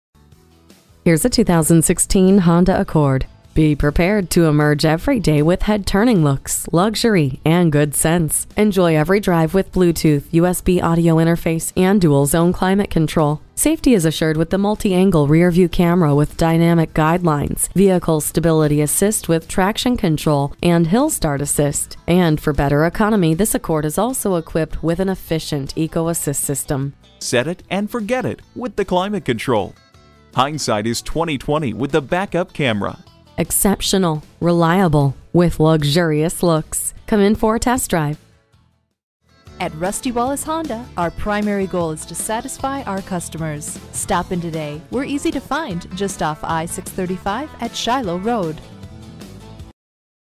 2016 Honda Accord Sedan Dallas TX Fort Worth, TX #160167 - SOLD
Year: 2016
Make: Honda
Model: Accord Sedan
Trim: 4dr I4 CVT LX
Engine: 4 Cyl - 2.4 L
Transmission: Variable
Color: Crystal Black Pearl
Stock #: 160167
VIN: 1HGCR2F34GA018667

Address:
12277 Shiloh Road